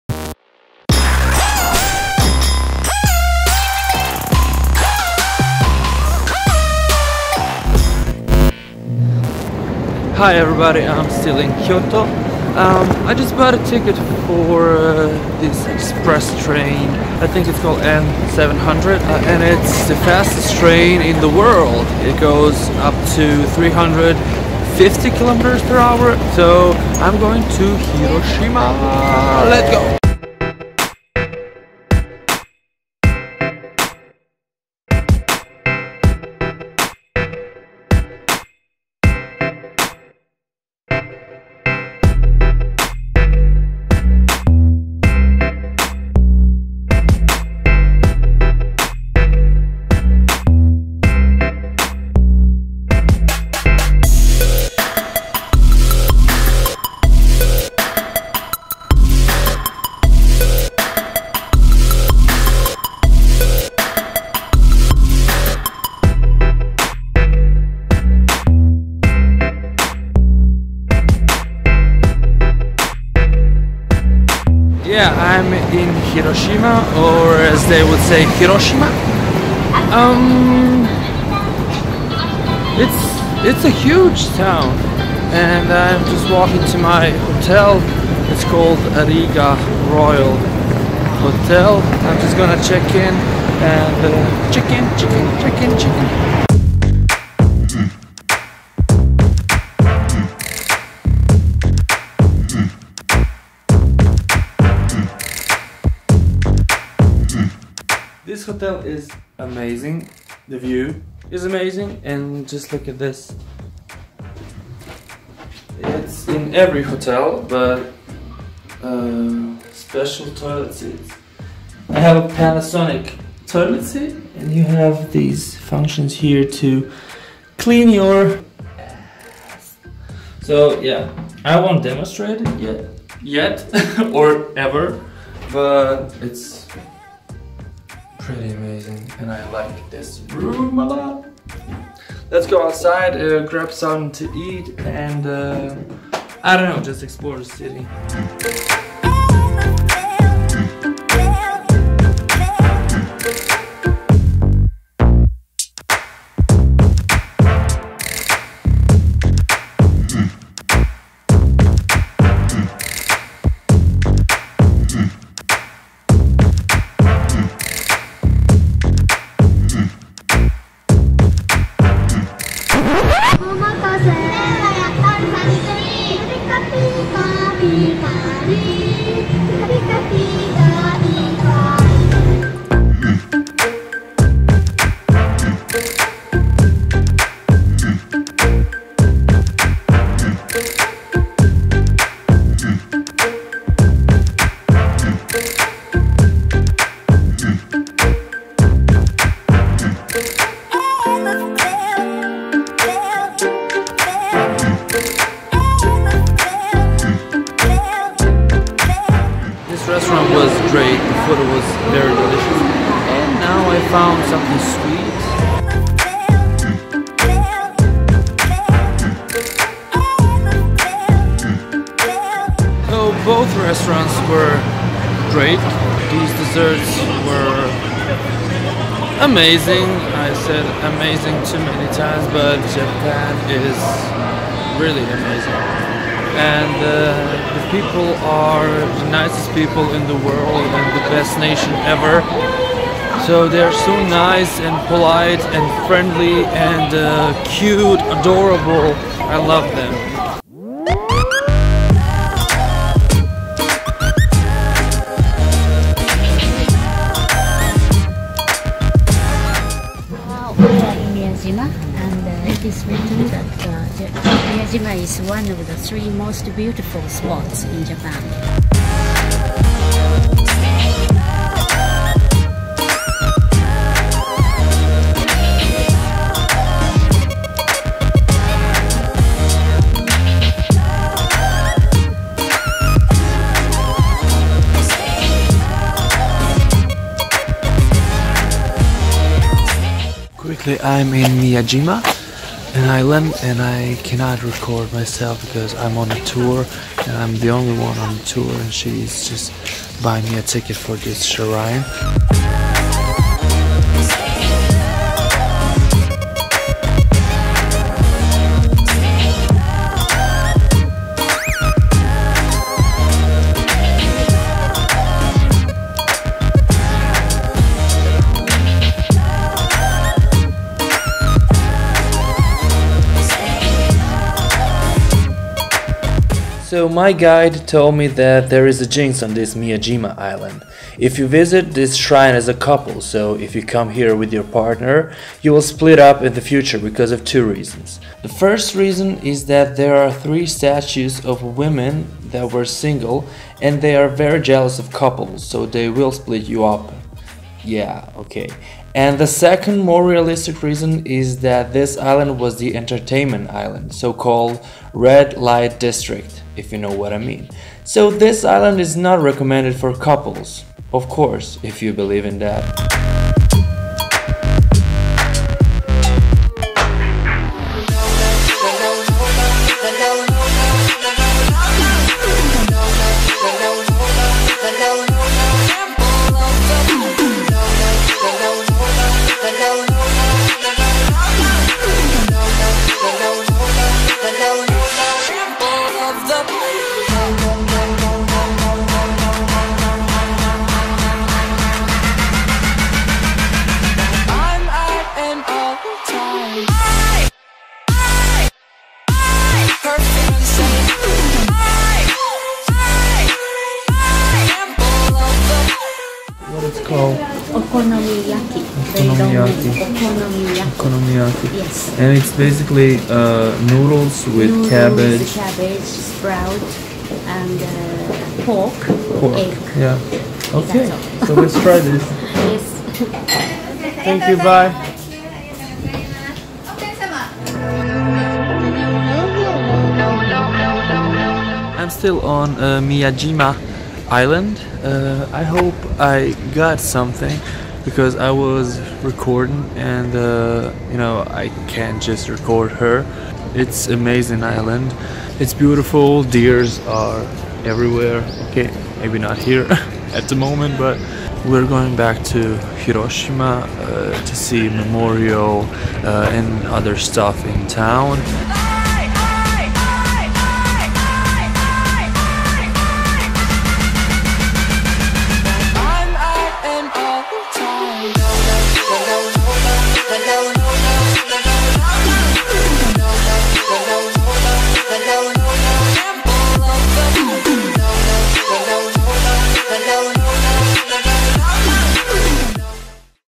MIYAJIMA ISLAND - NOT FOR COUPLES!!
Hi everybody, I finally made it to the amazing Hiroshima! Before the tour around the city, I went to the famous Miyajima (Itsukushima) island to see this famous orange Shinto shrine called the Itsukushima Shrine, of course. So, what is with this dramatic title you may ask? Well, there is a jinx about this island, just watch this vlog and find out ;)